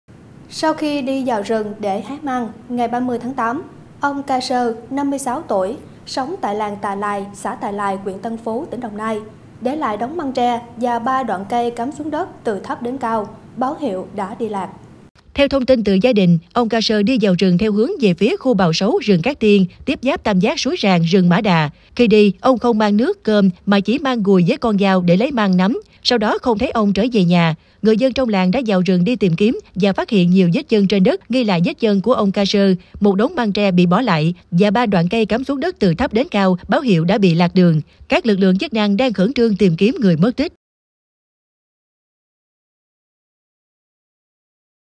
Tìm người đàn ông đi lạc gần 1 tuần trong vườn Quốc gia Cát Tiên
Sau khi đi vào rừng để hái măng ngày 30/8, ông Ka Sơr (65 tuổi, sống tại làng Tà Lài, xã Tà Lài, huyện Tân Phú, tỉnh Đồng Nai) để lại đống măng tre và 3 đoạn cây cắm xuống đất từ thấp đến cao báo hiệu đã đi lạc.